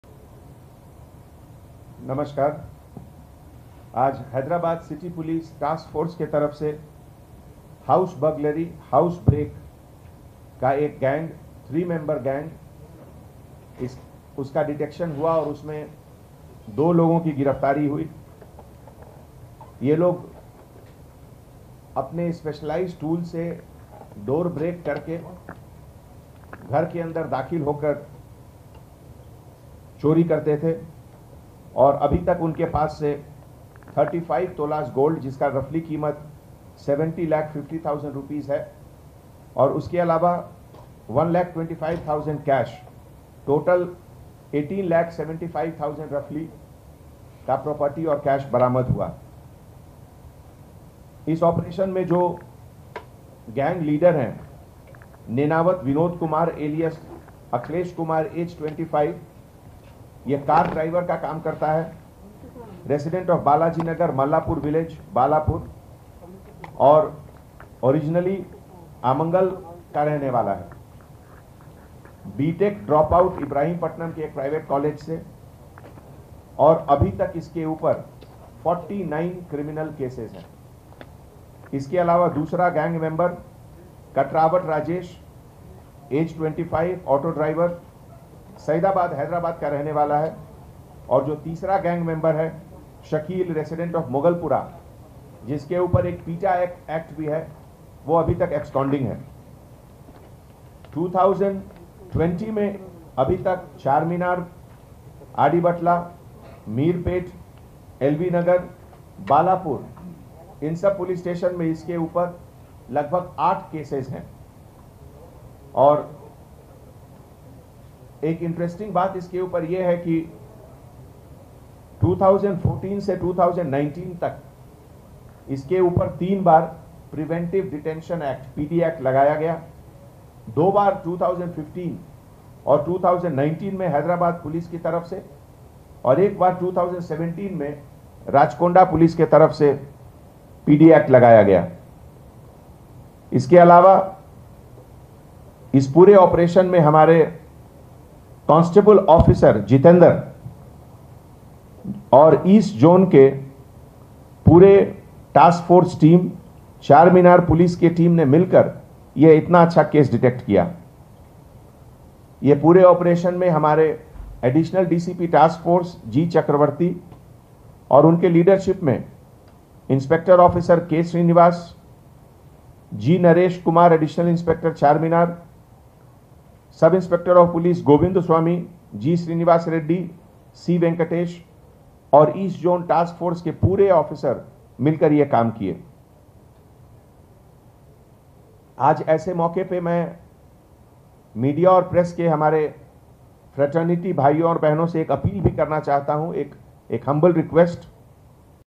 Press Conference Hyderabad City Commissioner Sri Anjani Kumar IPS
*2 HB offenders Vinod naik & Rajesh nabbed by Inspector eastzone taskforce Sri.Srinivas & team. Seized property worth 20 lakhs from their possession. City Police commissioner Sri.Anjani kumar IPS will address to media today at 12 noon in his office at basheerbagh. covered by THE WORLD EXPRESS